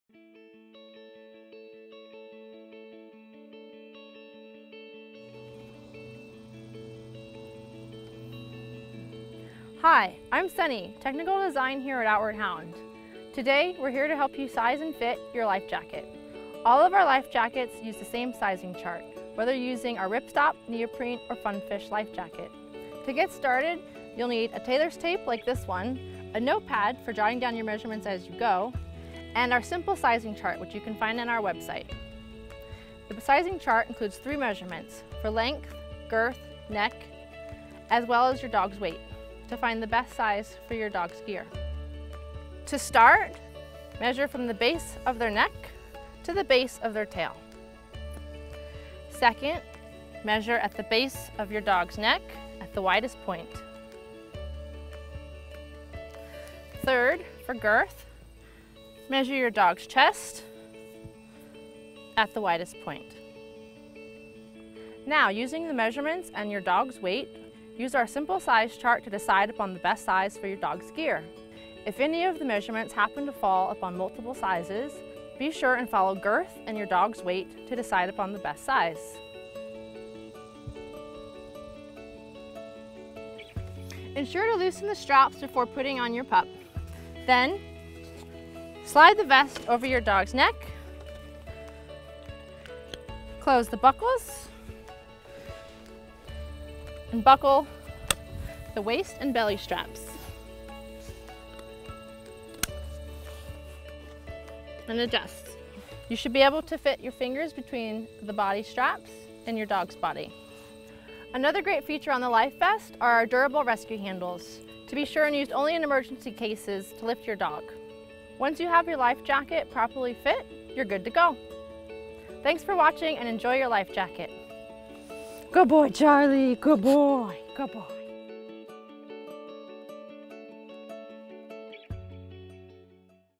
How To Size and Fit Your Outward Hound Dog Life Jacket
As pup parents ourselves, we know how hard it can be to find and fit the right size gear product for your dog. This video will help you size and fit your Outward Hound Life Jacket. All of our jackets use the same sizing chart, so the instructions will be the same whether you’re using our Ripstop, Neoprene or Fun Fish Life Jacket.